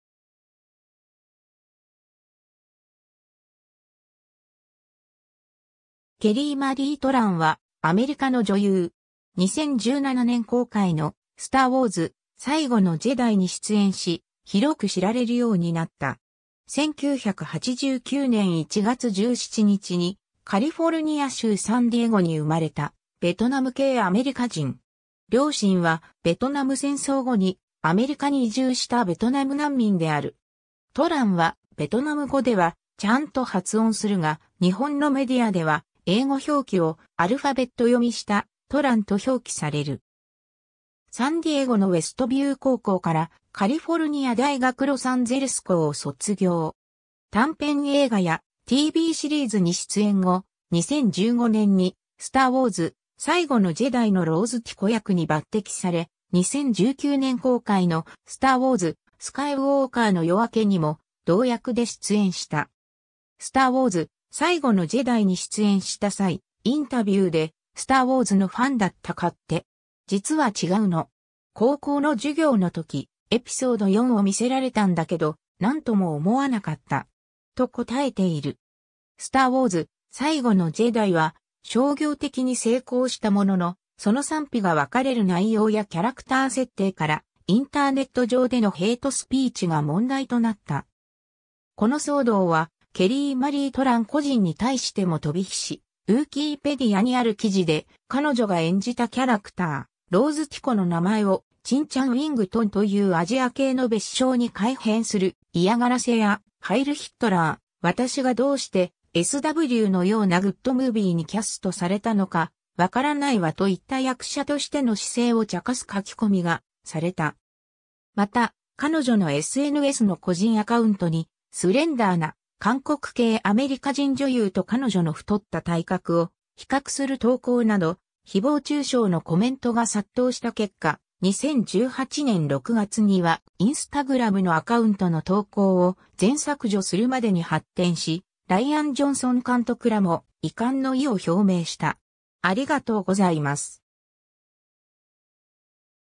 ケリー・マリー・トラン
ケリー・マリー・トラン（英: Kelly Marie Tran、越: Trần Loan / 陳鸞、1989年1月17日 - ）は、アメリカの女優。
 2017年公開の『スター・ウォーズ/最後のジェダイ』に出演し、広く知られるようになった。
 1989年1月17日にカリフォルニア州サンディエゴに生まれたベトナム系アメリカ人。
両親はベトナム戦争後にアメリカに移住したベトナム難民である。
 Tran（越: Trần）は、ベトナム語では「チャン」と発音するが、日本のメディアでは英語表記をアルファベット読みした「トラン」と表記される。
 サンディエゴのウェストビュー高校からカリフォルニア大学ロサンゼルス校 (UCLA) を卒業。
短編映画やTVシリーズに出演後、2015年に『スターウォーズ/最後のジェダイ』（2017年公開）のローズ・ティコ役に抜擢され、2019年公開の『スター・ウォーズ/スカイウォーカーの夜明け』にも同役で出演した。
 『スターウォーズ/最後のジェダイ』に出演した際、インタビューで「『スター・ウォーズ』のファンだったかって？実は違うの。高校の授業のとき『エピソード4』を見せられたんだけど、何とも思わなかった。」と答えている。
 『スターウォーズ/最後のジェダイ』は商業的に成功したものの、その賛否が分かれる内容やキャラクター設定から、インターネット上でのヘイトスピーチが問題となった。
 この騒動はケリー・マリー・トラン個人に対しても飛び火し、Wookieepediaにある記事で彼女が演じたキャラクター、ローズ・ティコの名前を「Ching Chong Wing Tong」というアジア系の蔑称に改変する嫌がらせや、「ハイル・ヒットラー、私がどうしてSWのようなグッド・ムービーにキャストされたのか分からないわ」といった役者としての姿勢を茶化す書き込みがされた。
 また、彼女のSNSの個人アカウントにスレンダーな韓国系アメリカ人女優と彼女の太った体格を比較する投稿など、誹謗中傷のコメントが殺到した結果、2018年6月にはインスタグラムのアカウントの投稿を全削除するまでに発展し、ライアン・ジョンソン監督らも遺憾の意を表明した。